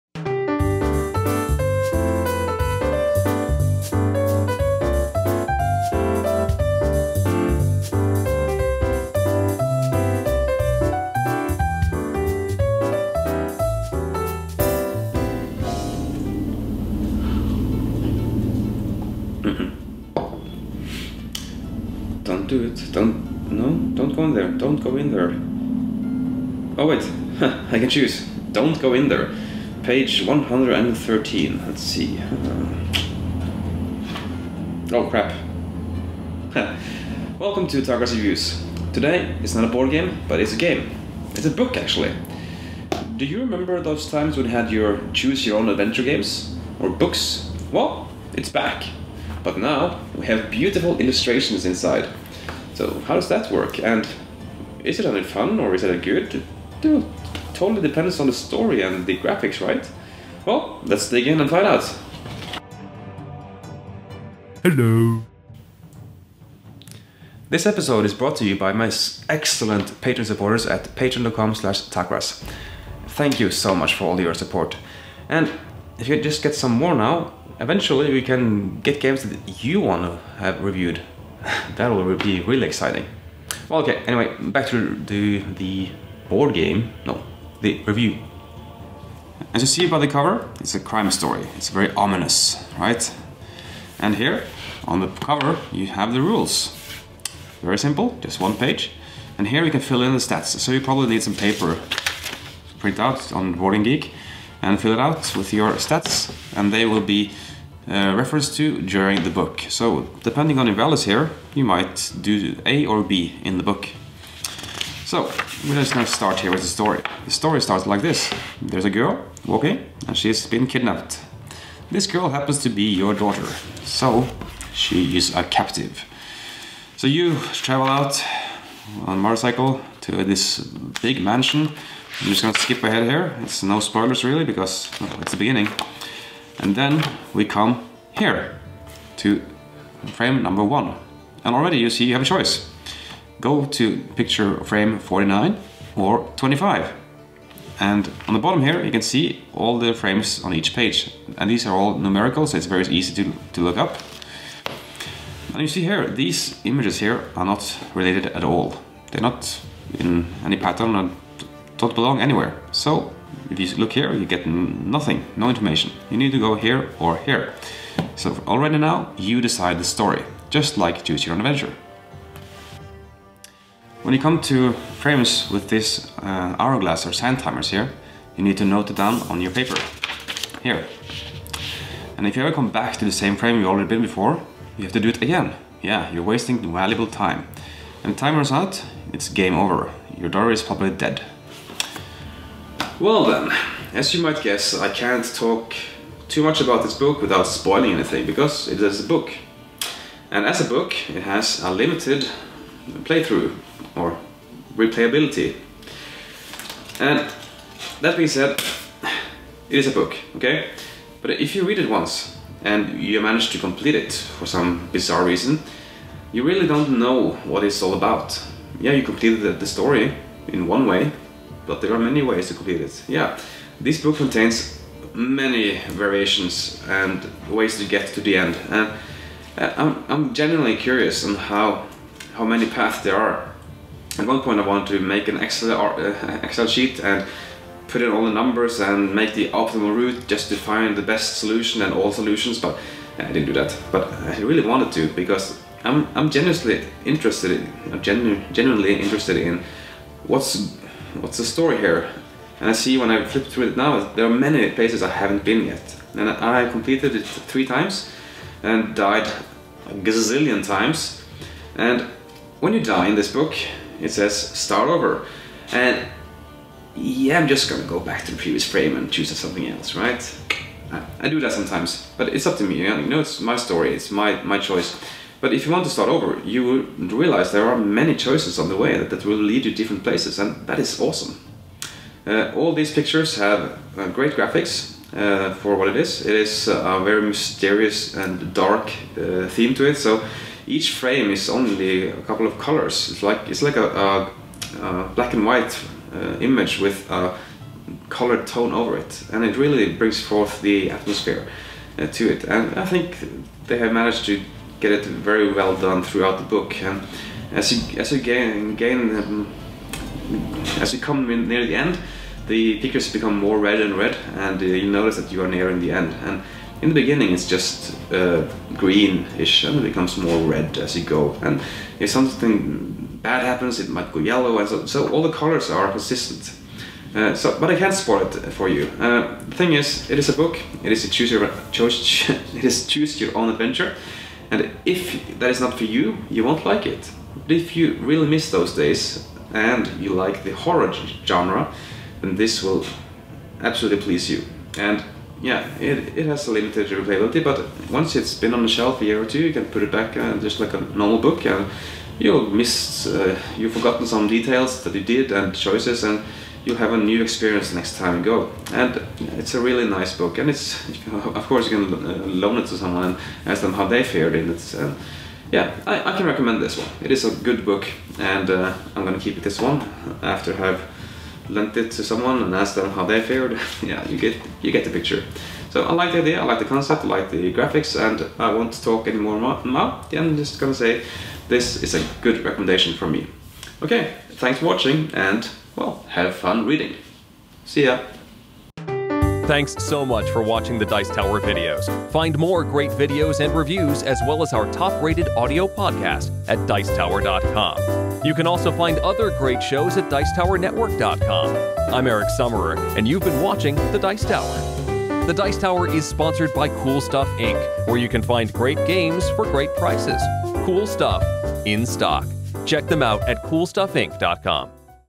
Captive Review - with André Nordstrand
André Nordstrand checks out this player-driven book adventure with a horror setting.